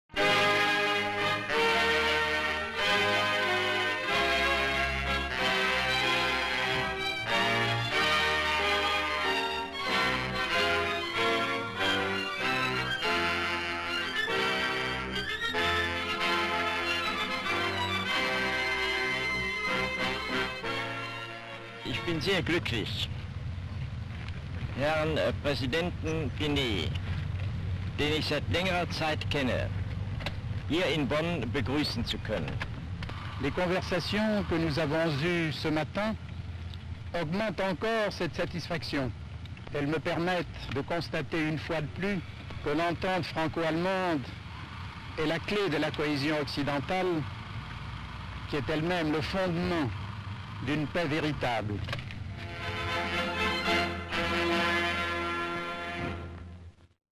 Franco - German Talks In Bonn (1955)
Franco-German talks in Bonn, Germany.

MS arrival of French Prime Minister Antoine Pinay at the Palais Shaumburg.  LS the German and French flags flying.  MS Pinay and Chancellor of Federal German Republic Konrad Adenauer.  MS group of cameramen.  CU Adenauer speaking into microphone in German (natural sound).  CU Pinay speaking into microphone.  Various shots Pinay speaking into microphone in French (natural sound), he speaks about the improvement relations between Germany and France.  MS Adenauer and Pinay shaking hands.

 FILM ID:2680.09